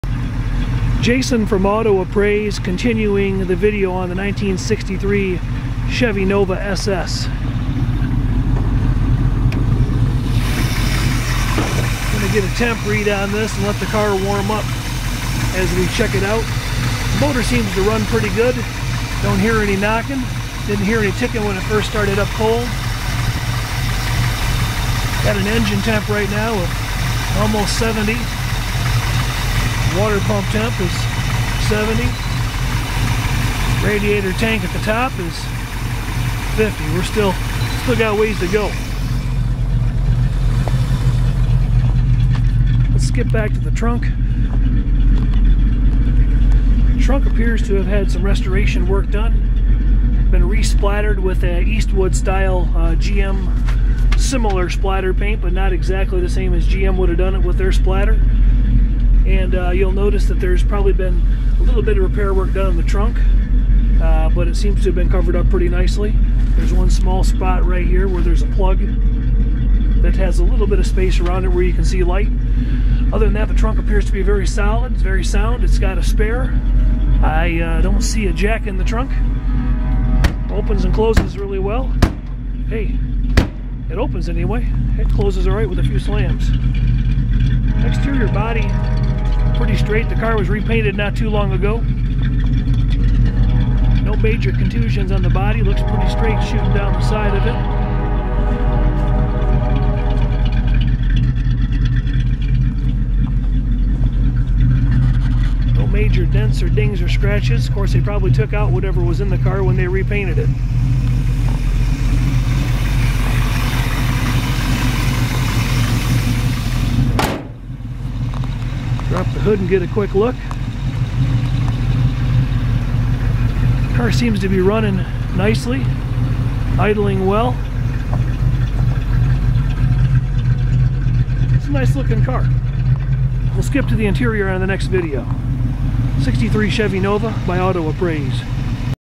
1963 Chevy Nova Test Drive 2, Auto Appraise
Auto Appraise, Inc. Nationwide Group of Certified Auto Appraisers, cars jacked up on site, 150+ pics, essay style reports, auto appraisals.  1963 Chevrolet Nova SS Video 2 - Hernandez.